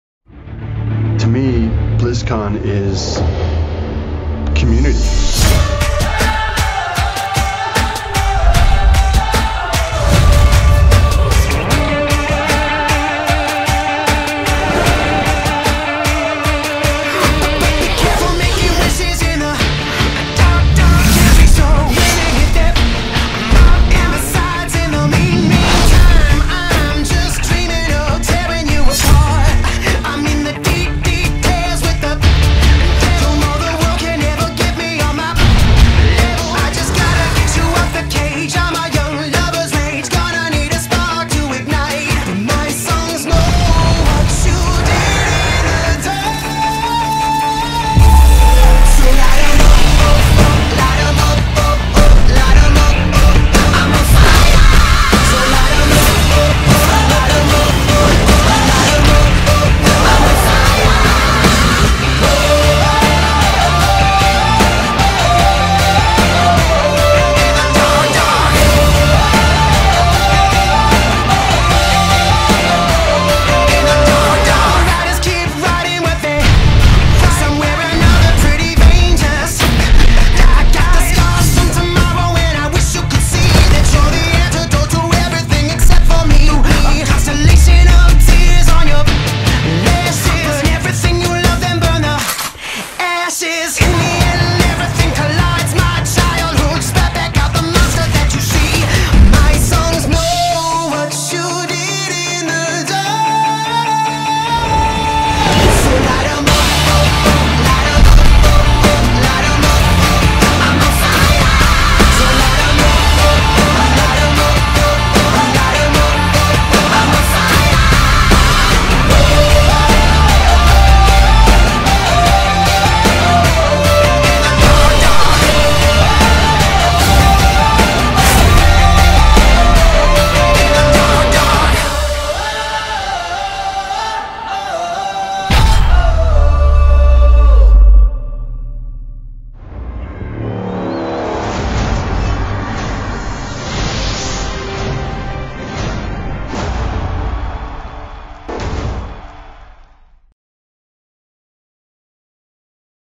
Edited Video: No Regret to be a Blizzard Gamer
The video is edited by a Chinese Blizzard games fan. The owesome scenes with passionate BGM totally fire me up